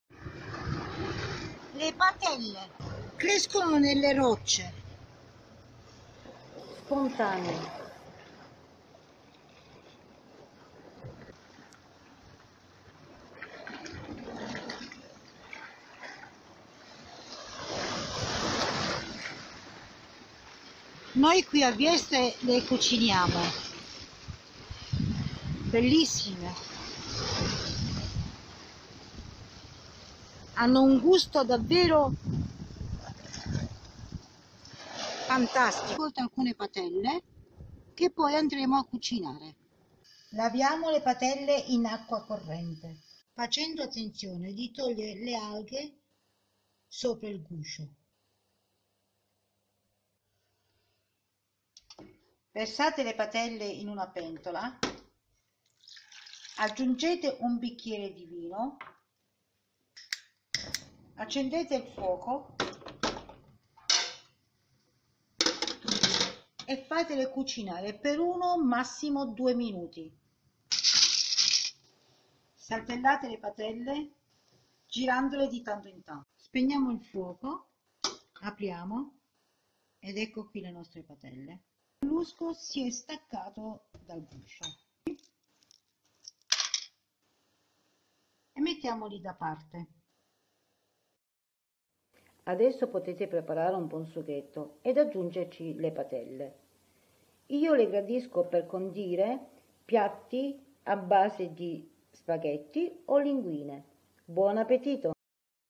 Come sgusciare le patelle ricetta pugliese facile e veloce
Passeggiando tra gli scogli di Vieste possiamo ammirare le patelle ovvero molluschi in conchiglia o frutti di mare che crescono liberamente lungo il Gargano come su tutta la costa pugliese. I turisti li vedono disinteressati senza conoscere la cucina locale e la semplicità della ricetta come sgusciare le patelle in modo facile e veloce. Essendo originaria di Vieste fin da piccola ho imparato a mangiare questo tipo di frutti di mare a crudo ed apprezzare la cucina del Gargano in un tradizionale primo piatto come solo questo tipo di molluschi in conchiglia offre. Nel video vi mostro la ricetta pugliese come sgusciare questo tipo di frutti di mare impiegando 3 minuti in preparazione, 2 in cottura e 5 in separazione della conchiglia ai molluschi. Qui a Vieste ed in Gargano amiamo la cucina locale tradizionale offrendo il lascito dei nostri antenati. Fate la prova, seguite passo dopo passo il video e vedrete voi stessi quanto facile e veloce sia preparare un gustoso primo piatto per voi ed i vostri ospiti.